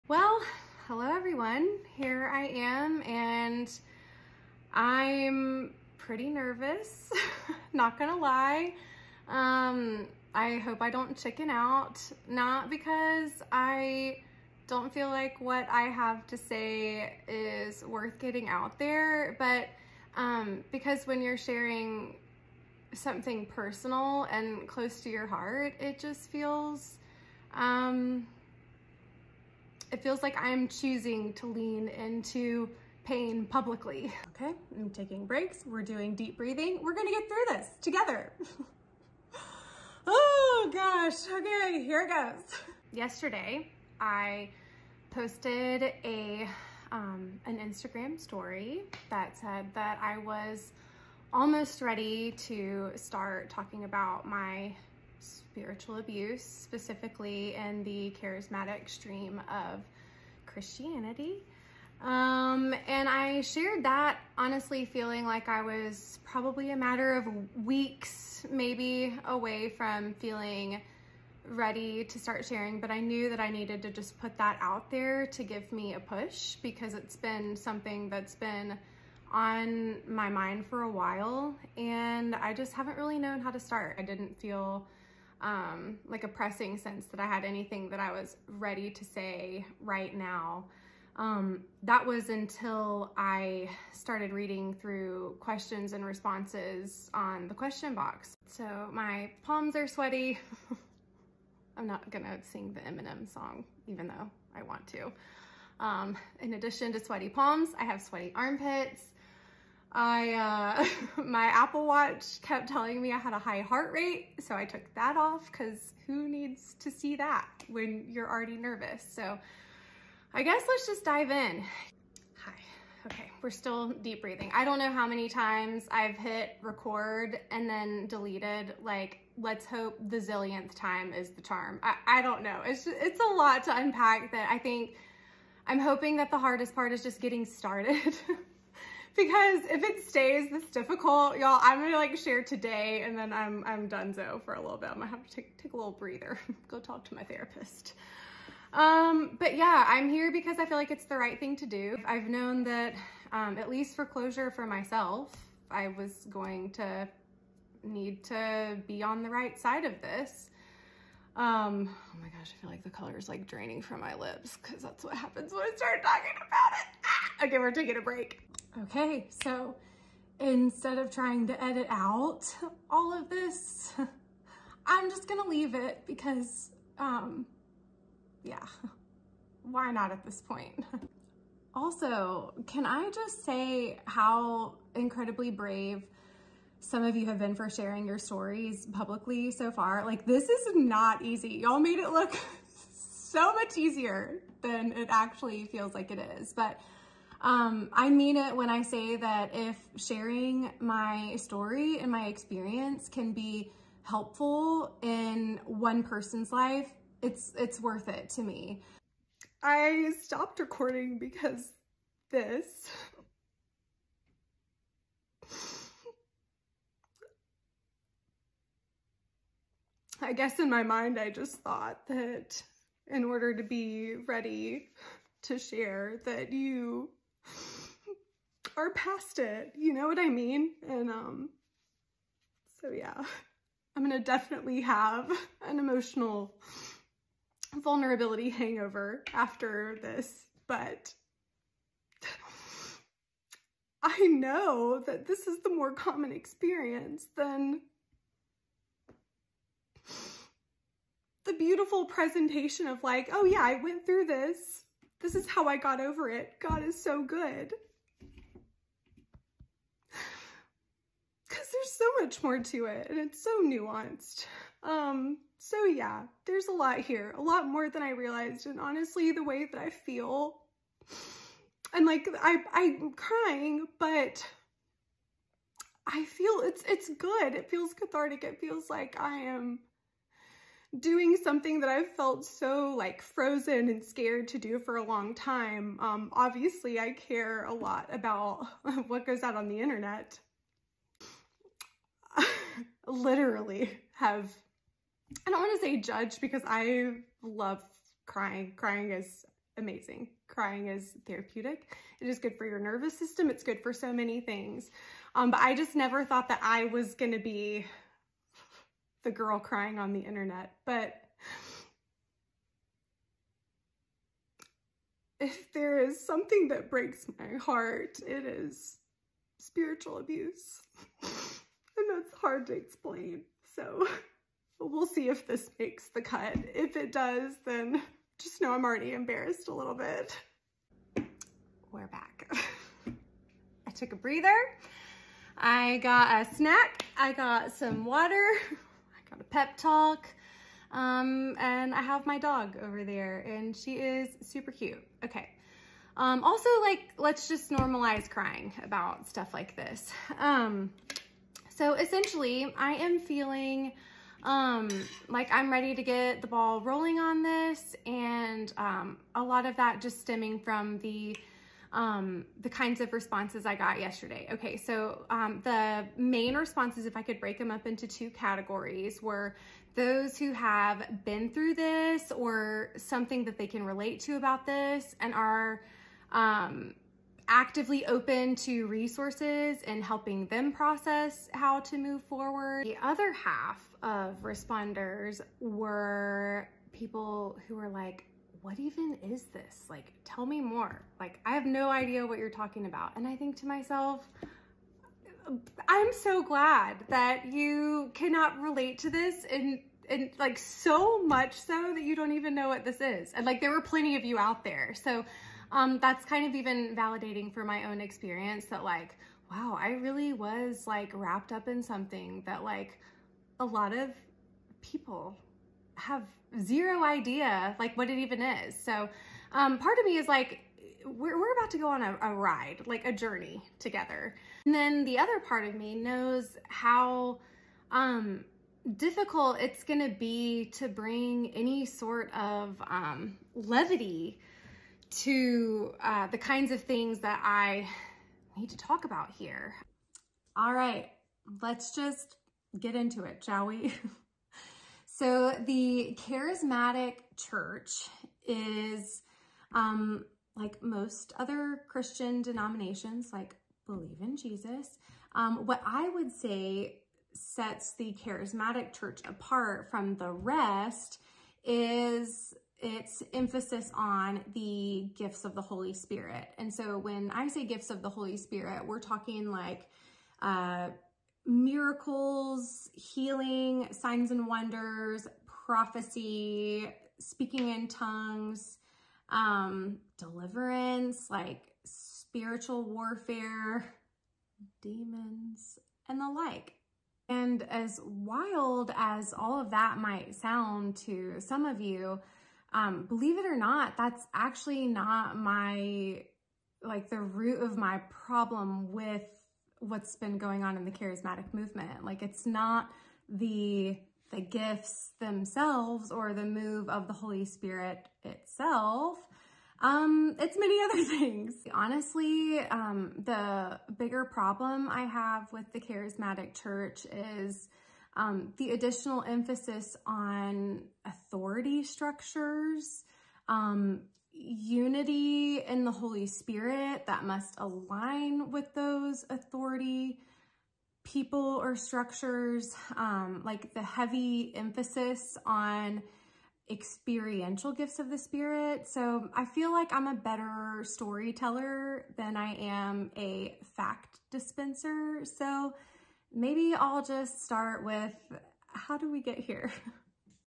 Charismatic Spiritual Abuse: Part 1
***In this first video, I do not make it very far in terms of CONTENT and storylines, but I left it to be able to document how difficult it was to begin discussing the issue altogether. For those who are looking for educational tools or storylines, this might not be for you. For those of you who have taken the time, thank you for listening with care.***

Finally breaking the silence and ready to start talking about charismatic spiritual abuse. For context, not all charismatic movements are the same. Specifically, we were wrapped up in more of a “hyper-charismatic” stream of Christianity.

There are healthy charismatic communities, and we have since been able to find healing, healthy leaders, and healthy, balanced communities. However, our journey had a rocky start, and it took years to find healing.

For those who were following along with our love story from the “Beth & Ryan—When God Wrote Our Love Story” videos, we are not finished sharing but we want to start all over.

We took down the initial videos because we were in the thick of our spiritual abuse at the time of posting and do not want to in any way promote hyper-spirituality or any form of striving or charismatic legalism.

Here’s a short update on us: We are still happily married—7 years next June. We are absolute best friends and still wake up every day amazed that God brought us together in such a detailed way. We have two beautiful twin toddlers and an adorable goldendoodle named Piper.

Thank you for taking the time to listen and care. We did not want to leave you guys hanging but could not just pop back on and share the “good stuff” without sharing about the excruciating pain in our walks with the Lord we were also experiencing during that time.